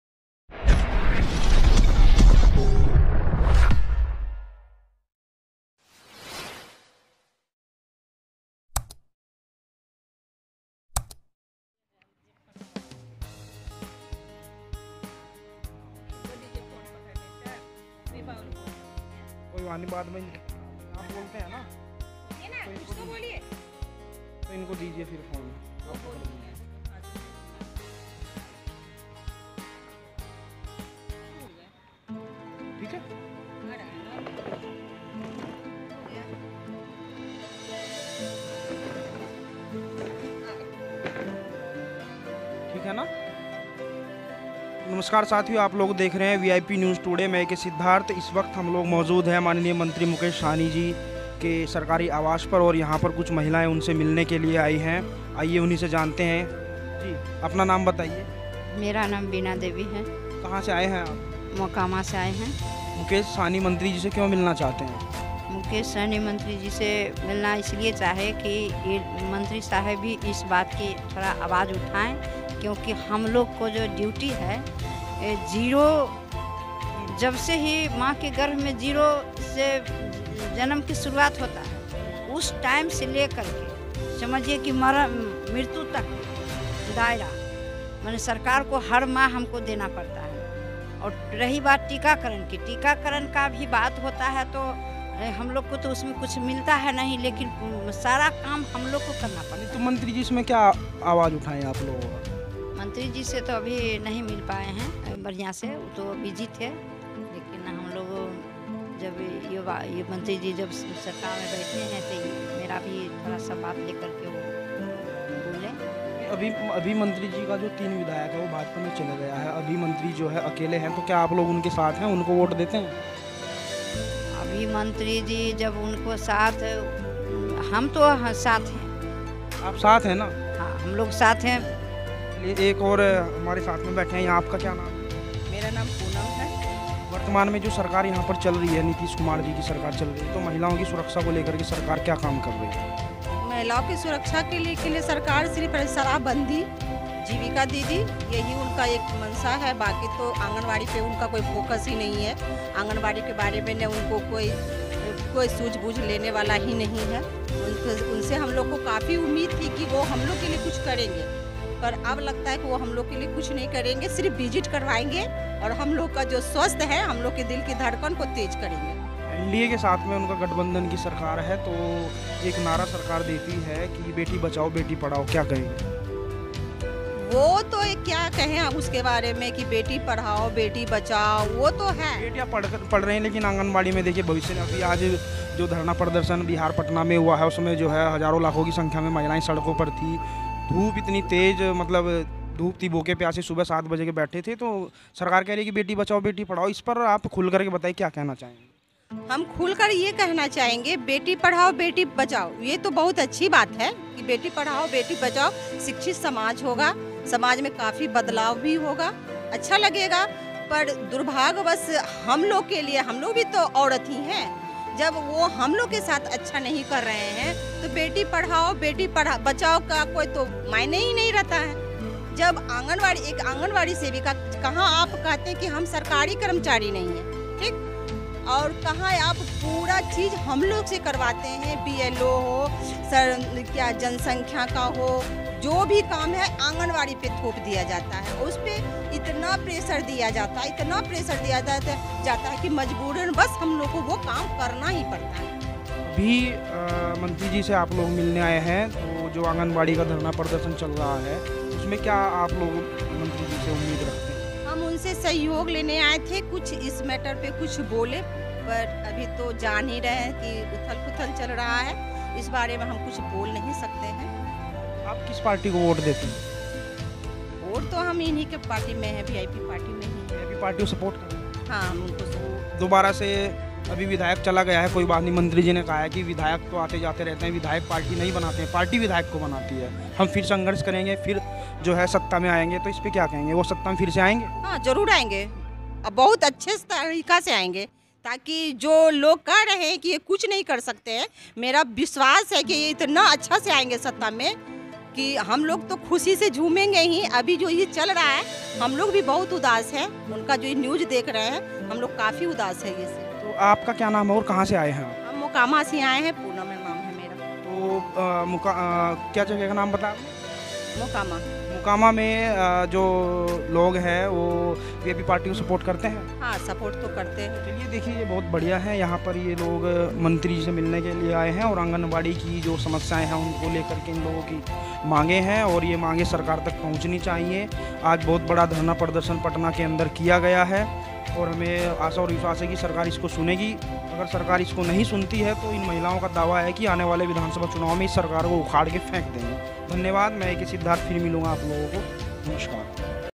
Bihar mein Mukesh Sahani hi jitenge अभी 3 विधायक भाजपा में गया है अब मुकेश सहनी 30 विधायक बनाएंगे
VIP News Today को आर्थिक रूप से मजबूत करने के लिए कृपया हमें मदद करें! Thanks

Phonepe
Google Pay
Paytm